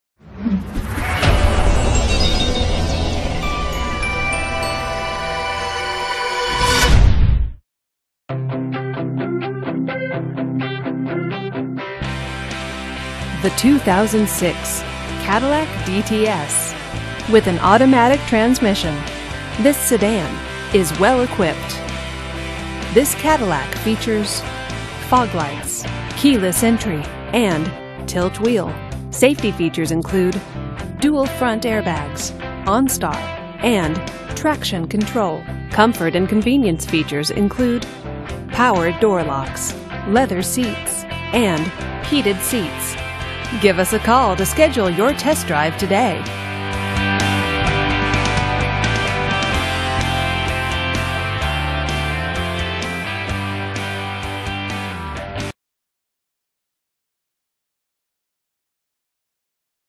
2006 Cadillac DTS Lubbock,Plainview,Midland,Odessa,Clovis 6U193178
2006 Cadillac DTS

Carlisle Motors
4302 Avenue Q

CARLISLE MOTORS, PROUDLY SERVING LUBBOCK, PLAINVIEW, MIDLAND, ODESSA, CLOVIS, AND SURROUNDING TEXAS LOCATIONS THIS GRAY 2006 Cadillac DTS IS EQUIPPED WITH A 8 Cylinder Engine 4.6L/279, AUTOMATIC TRANSMISSION, AND RECEIVES AN ESTIMATED 17 City/25 Hwy MPG.